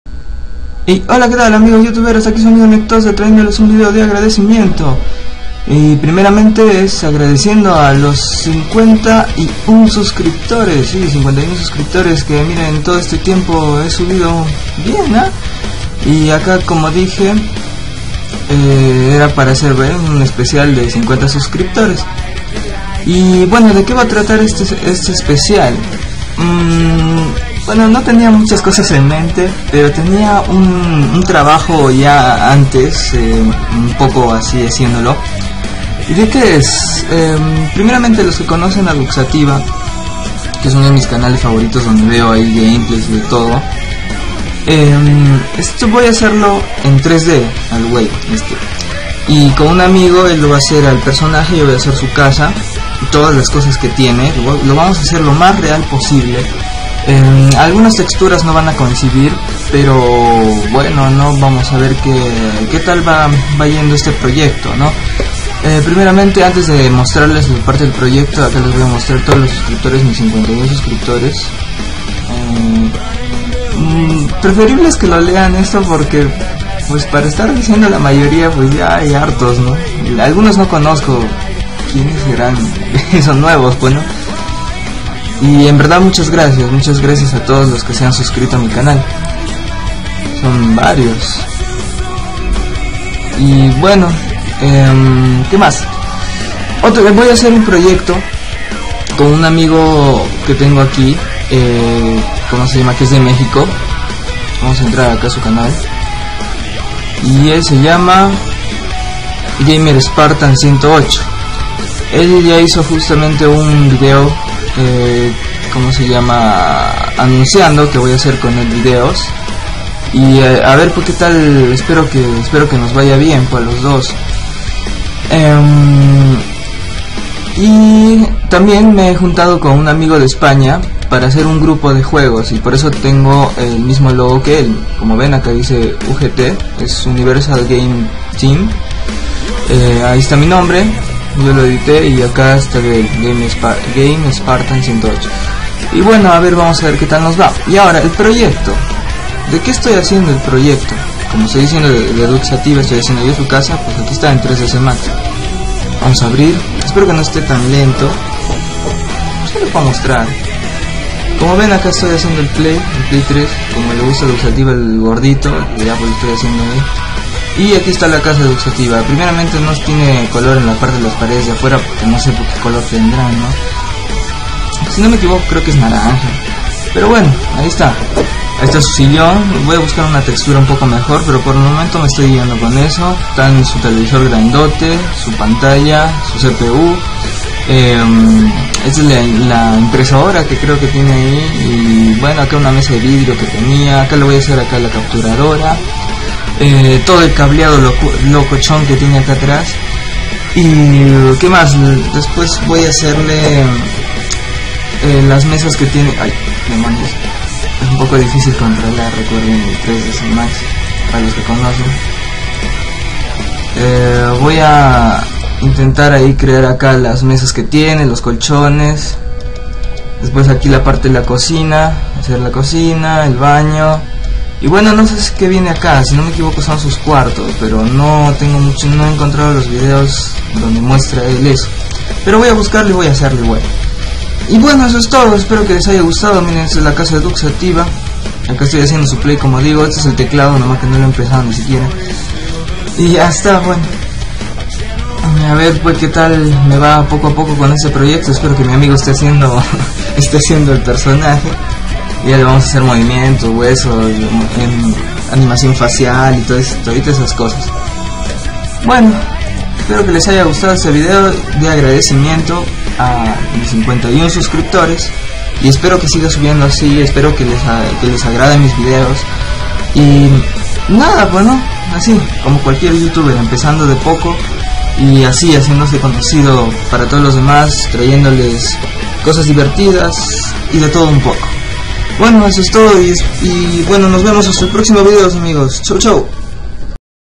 VIDEO AGRADECIMIENTO 51 SUSCRIPTORES, GRACIAS A TODOS...
video agradecimiento a todos mis 51 subs a mas, gracias a todos por ver mis videos y cada dia mejorare mas y mas

aqui estan mis paginas para que me puedan seguir: